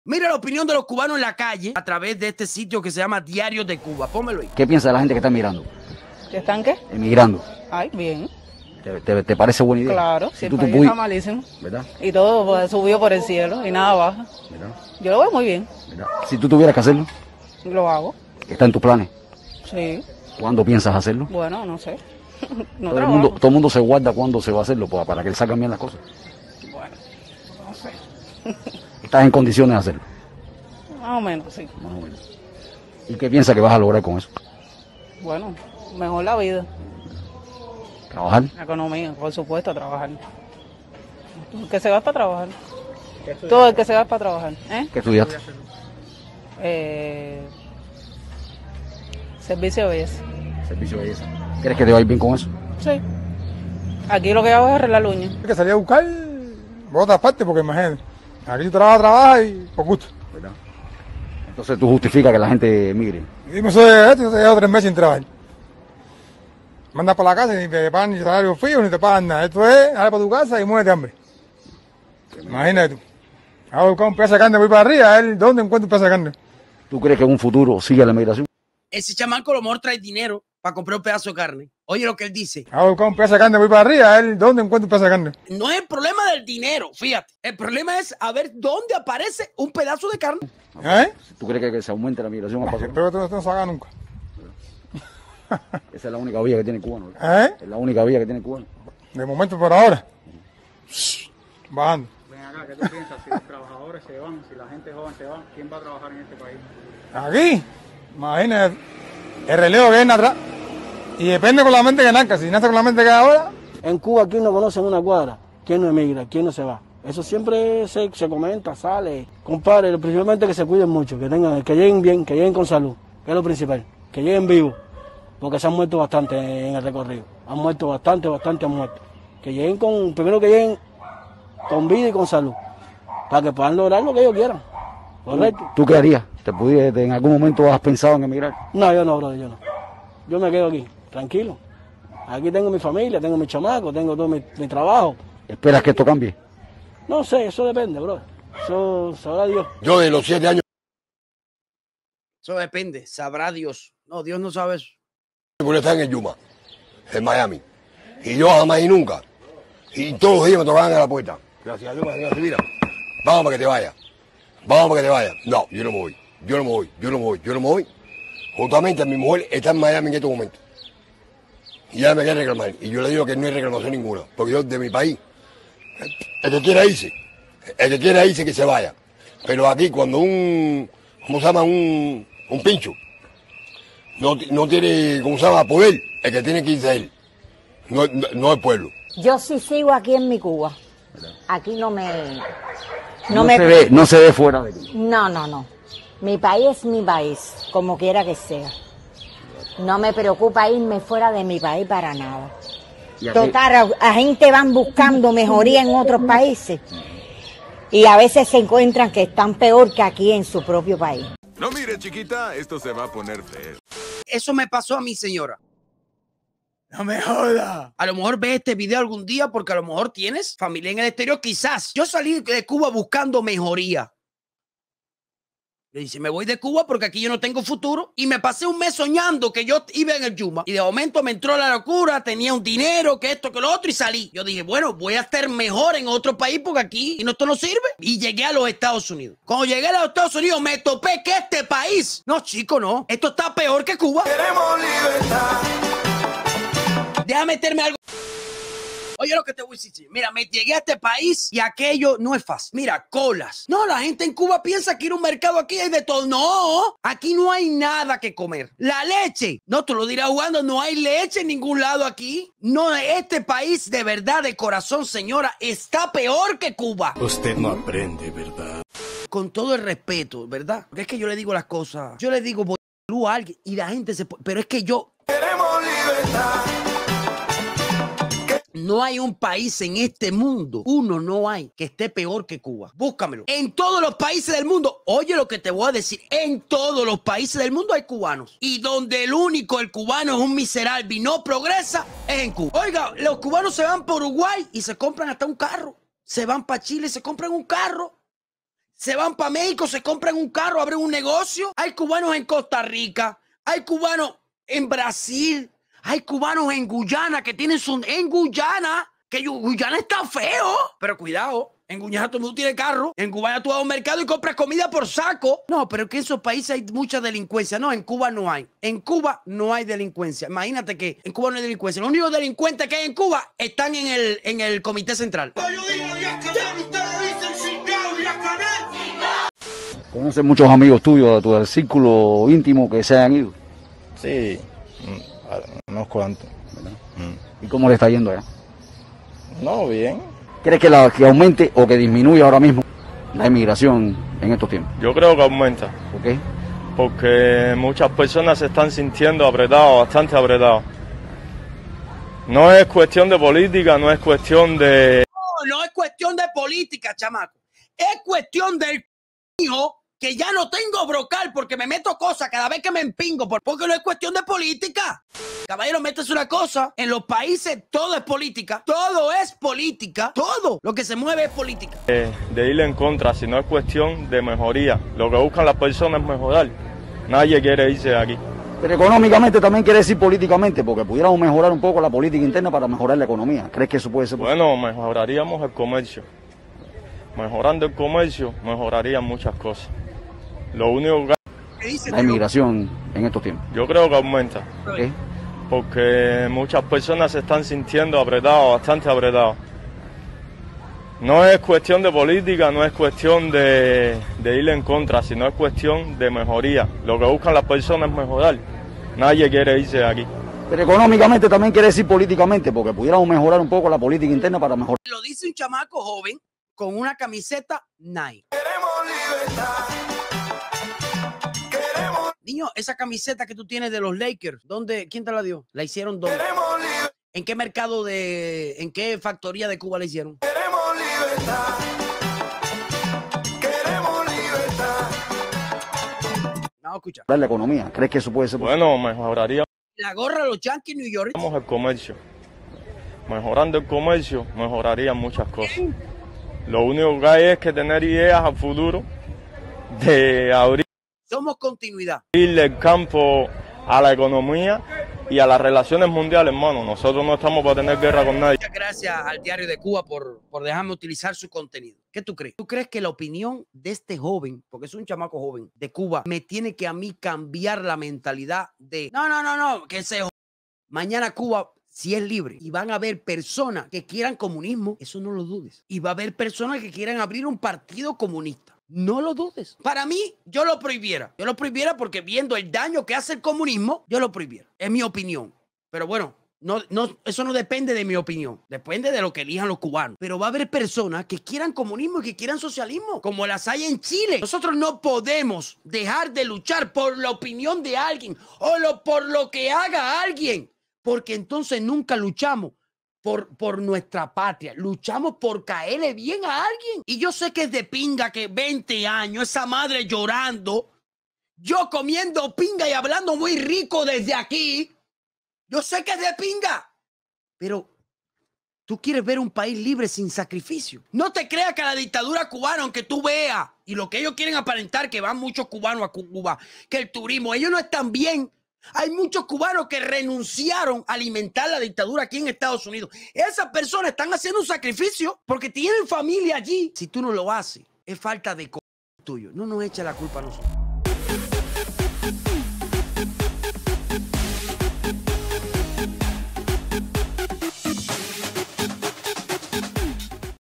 "Se van, y están peor que en CUBA" 😱 Reaccionando a los ¿Por qué los emigran desde Cuba?
Echate esta talla👇
click para ver más

Ultrack LLC